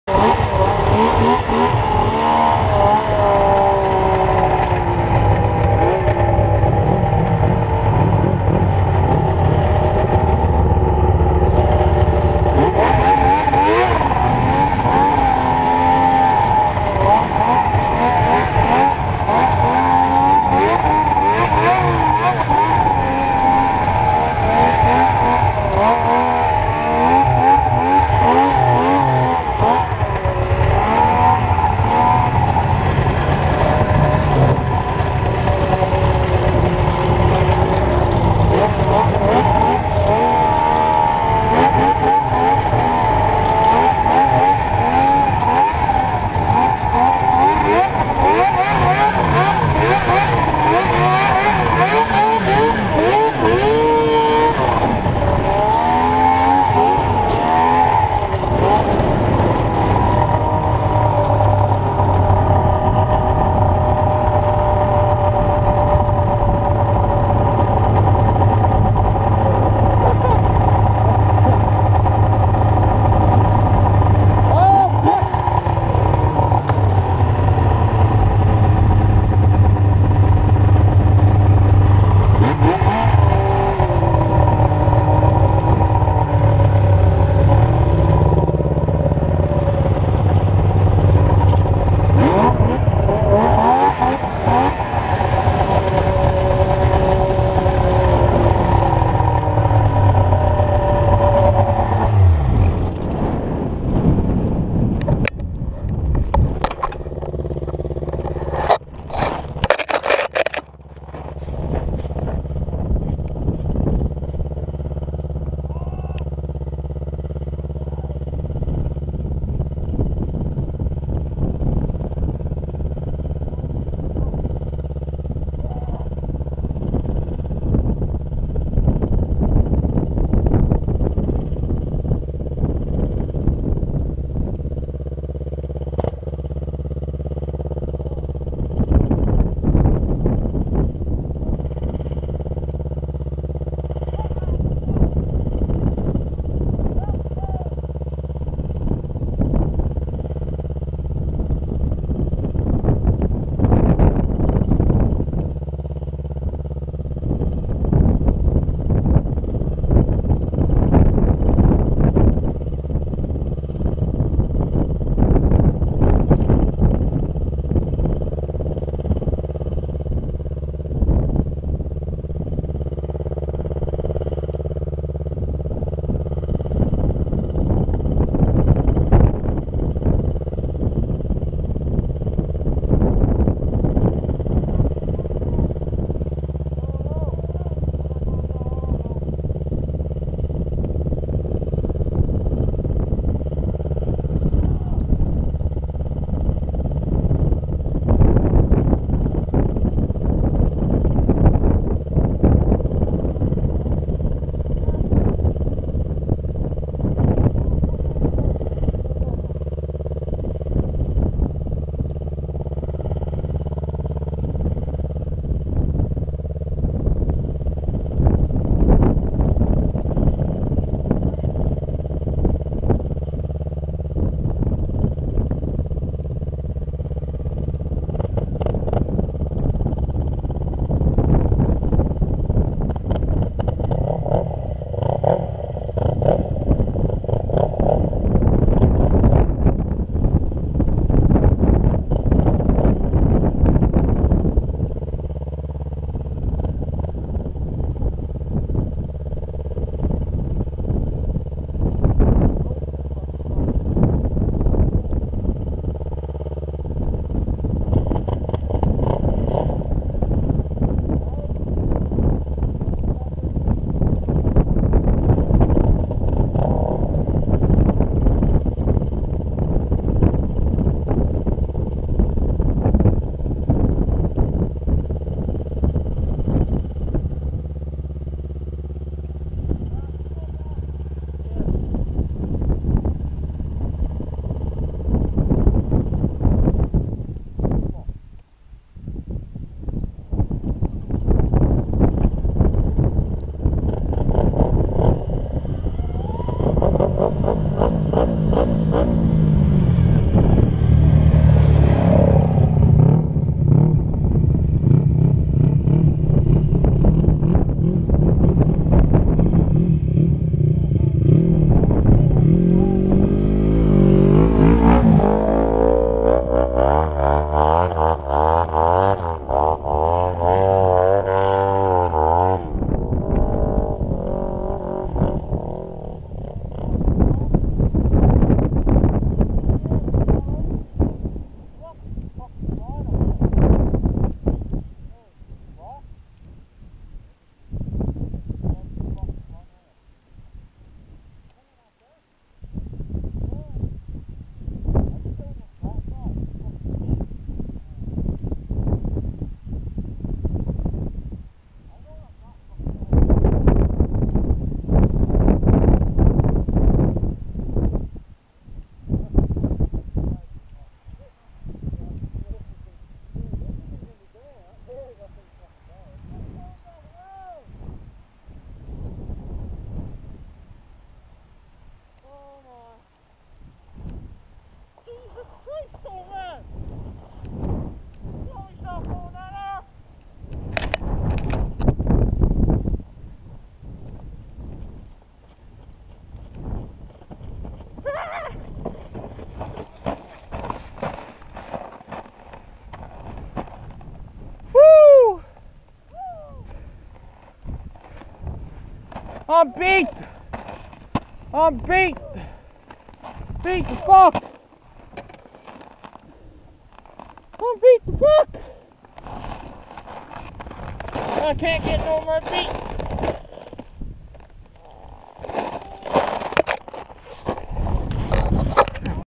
weens gets stuck so i gota climb this damn icy hill
Shes just about vertical and nutn but the sheet of ice